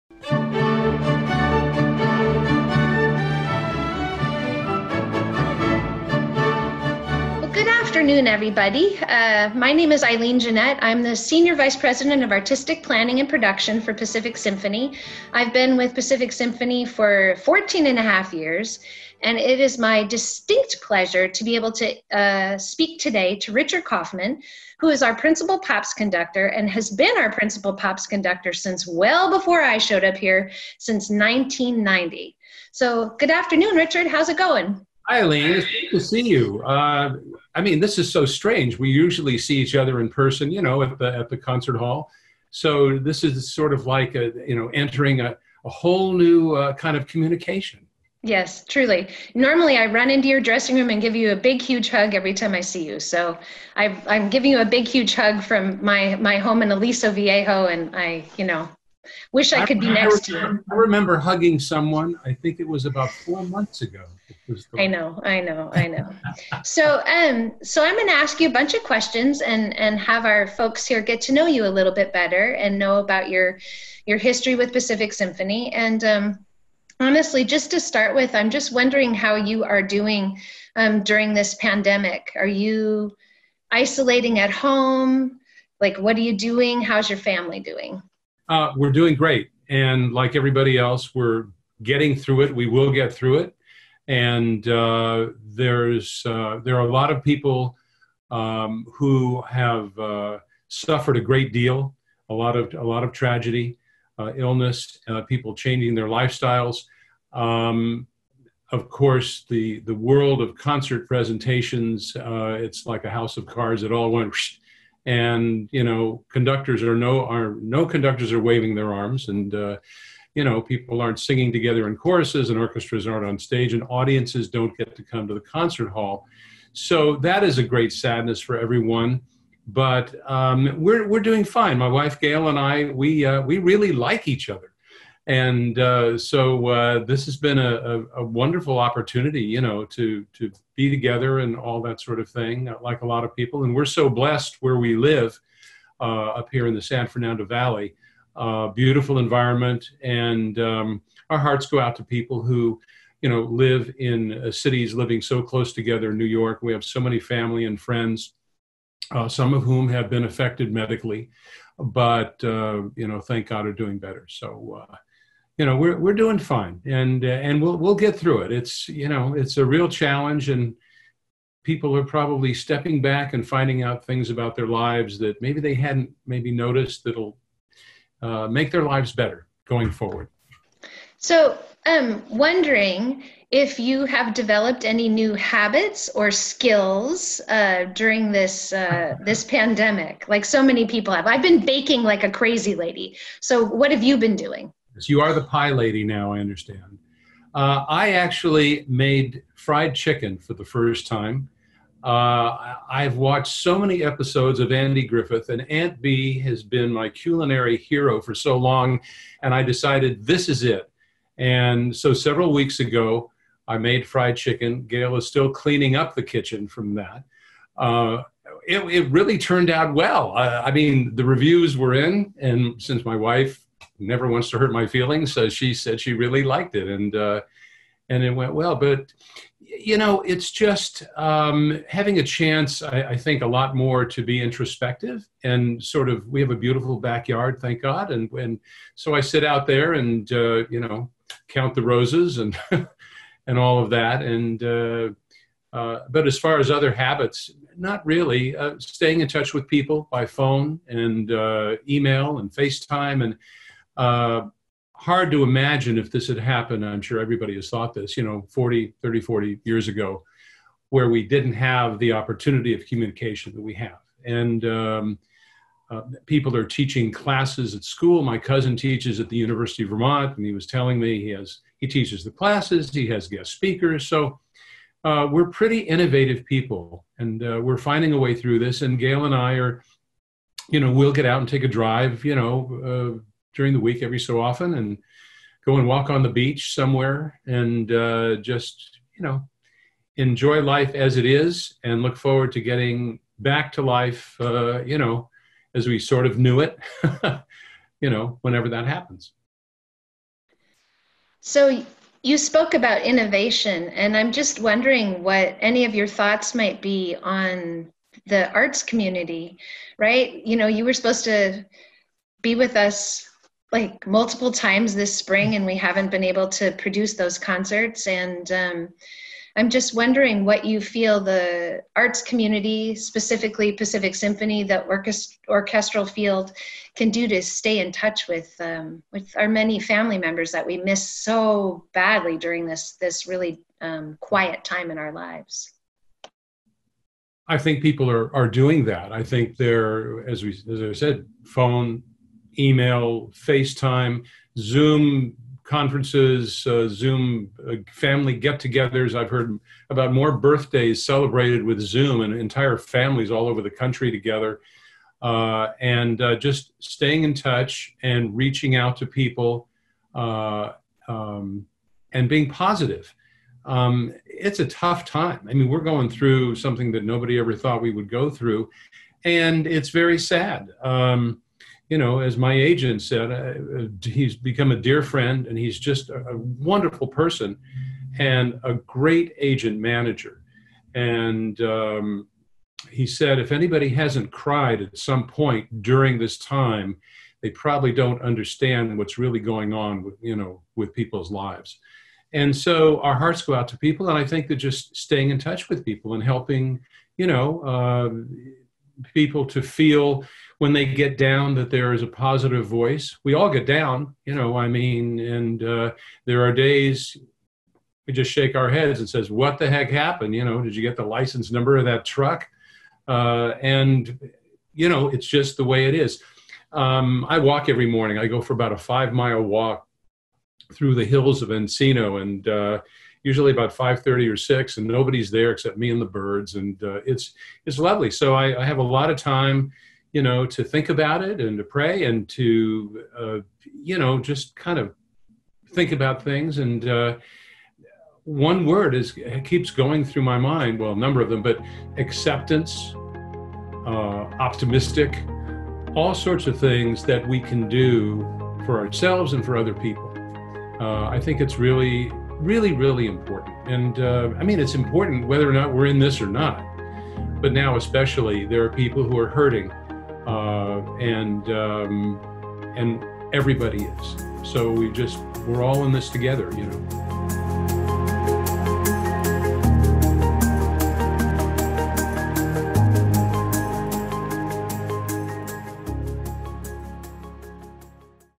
Stories with Richard Kaufman Part I: Introduction
Principal Pops conductor Richard Kaufman sat down for a conversation with Eileen Jeanette, our senior vice-president of artistic planning, for a 3-part conversation focusing on Richard's origins in Hollywood and his time with Pacific Symphony.

In this first episode, Richard discusses how he's handling the pandemic lock-downs, the importance of family time, and how the arts can stay innovative to stay ahead of the curve during this period.